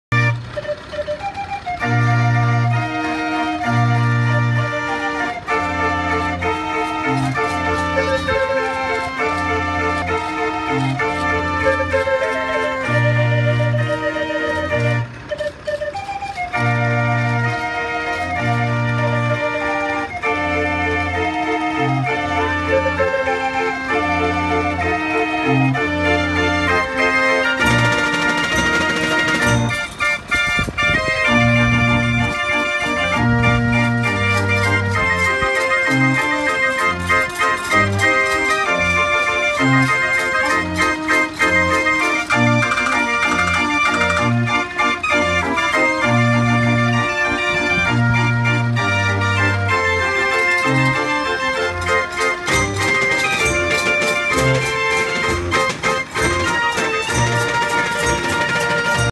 Voor het eerst een draaiorgel gezien
Voor het eerst in je jonge leven, komt er een draaiorgel voorbij. Dat is een vreemd muziekding. Nu gemotoriseerd. Vroeger werd de muziek handmatig gedraaid. Het orgel werd voortgetrokken door paarden.  Wat vind het erg leuk.